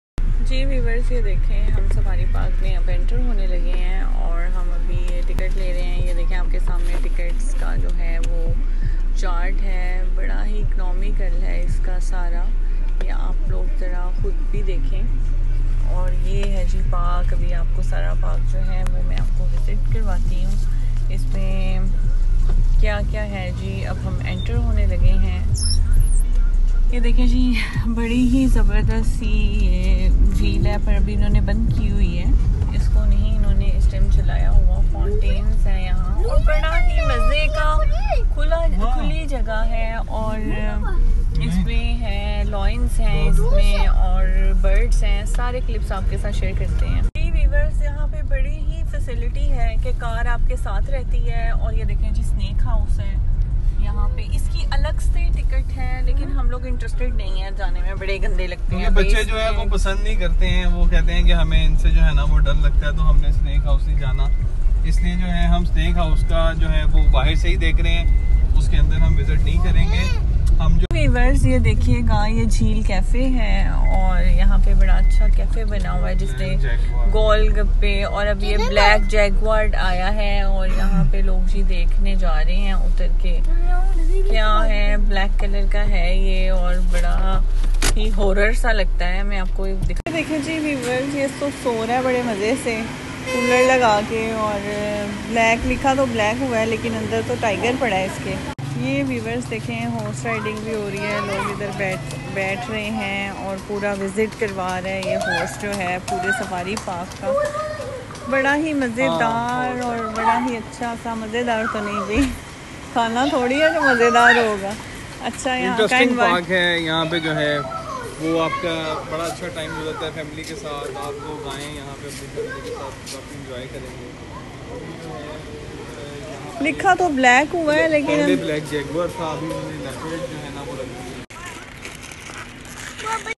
Lahore Safari Park Part-1
Visit to Lahore safari park with family